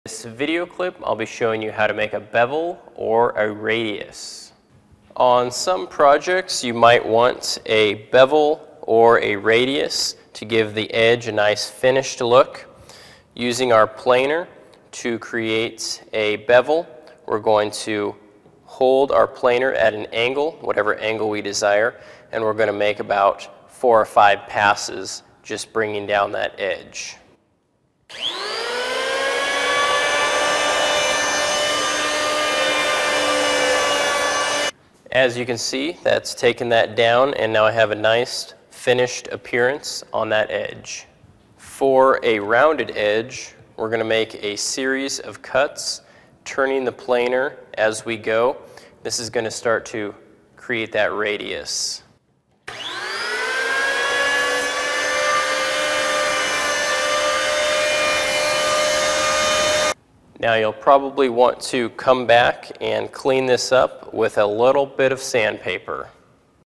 Creating Bevel With Power Planer
Creating Bevel With Power Planer. Part of the series: How to Use a Power Planer. Learn how to create a bevel or radius for a power planer and get professional tips and advice from an expert carpenter on using tools for leveling, shaving and shaping wood in this free woodworking video.